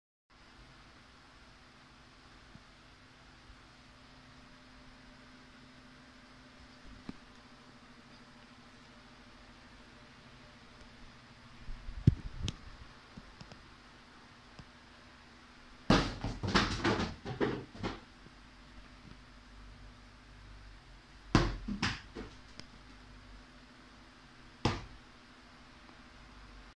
angry birds in real life?
So yeah... I made this set using vinyls made by spinmaster and some crocheted pigs.. Will i topple the piggy tower?